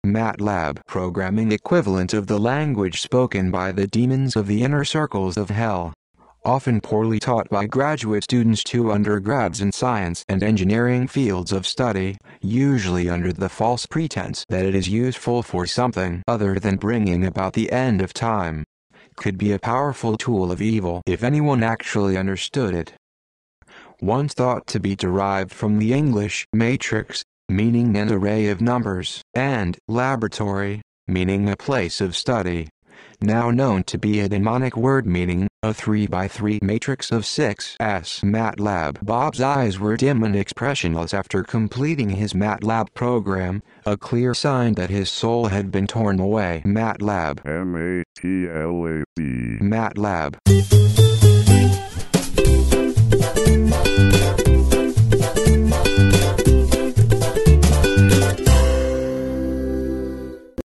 matlab meaning and pronunciation
Programming equivalent of the language spoken by the demons of the inner circles of Hell.  Often poorly taught by graduate students to undergrads in science and engineering fields of study, usually under the false pretense that it is usefull for something other than bringing about the end of time.  Could be a powerful tool of evil if anyone actually understood it.

Once thought to be derived from the English "matrix", meaning an array of numbers, and "laboratory", meaning a place of study.  Now known to be a demonic word meaning 'a 3x3 matrix of 6's'.

matlab definition by Urban Dictionary